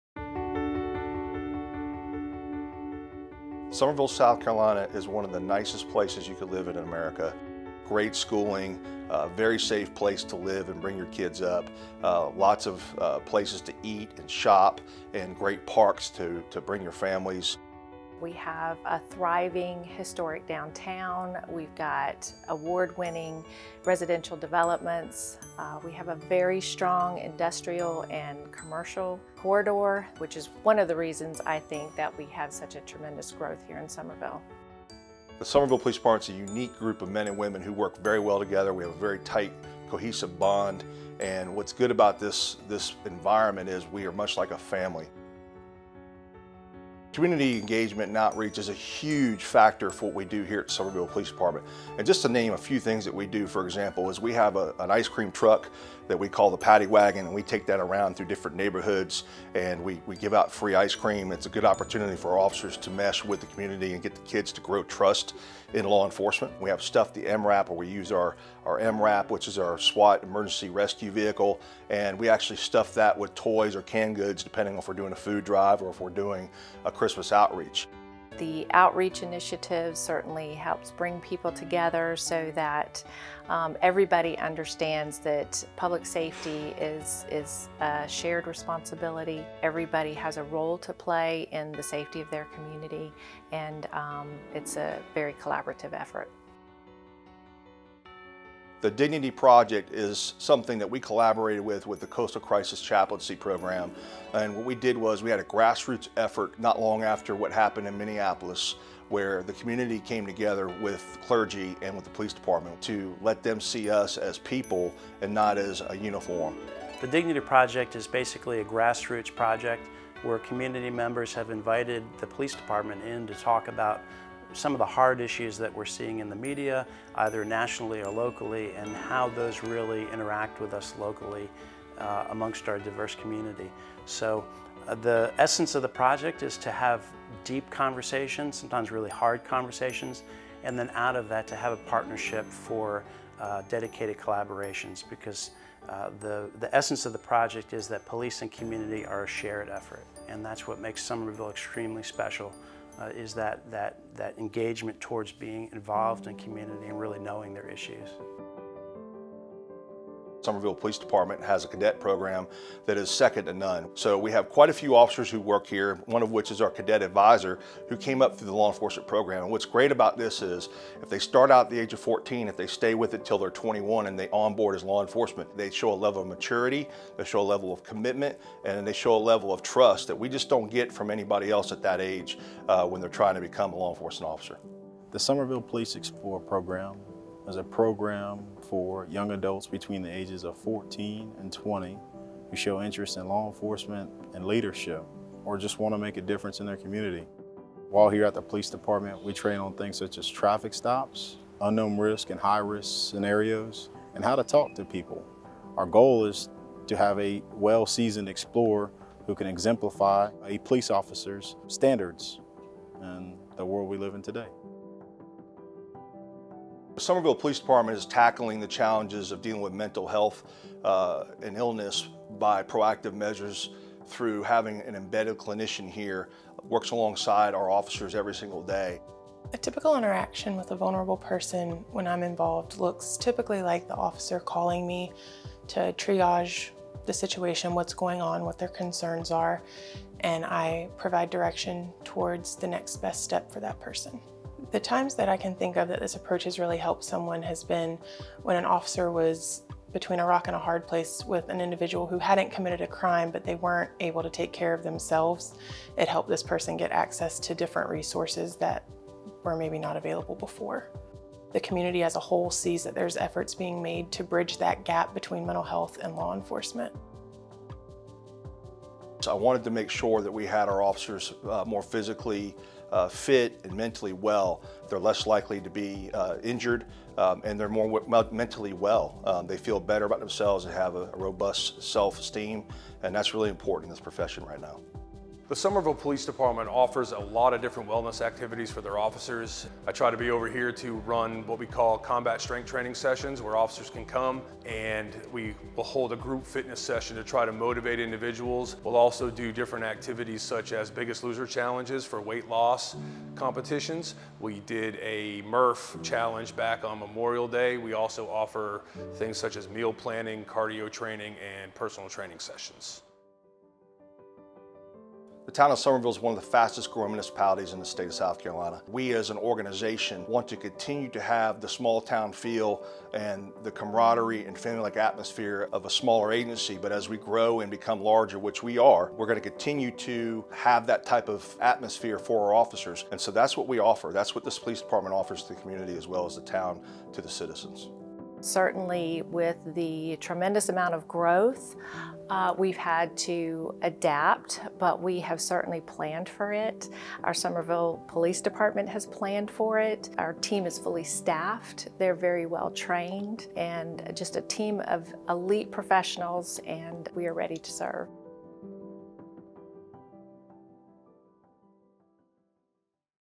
Summerville Police Department - SC
South Carolina’s Summerville is a great place to live with a vibrant friendly community, and Summerville Police Department is at the heart of this. Their goal is and has always been to help the community thrive by providing as many possible ways to interact with the community positively. Summerville’s Dignity Project has been key in ensuring the community’s voice is heard, and the police department’s approach reflects this.

Through community meetings to discuss problems, working with mental health professionals to ensure the right kind of intervention with the mentally ill, new non-lethal technology for use where force is necessary, as well as fitness and wellness training for all officers, Summerville PD is one of the most active departments in the state as well as the country within its community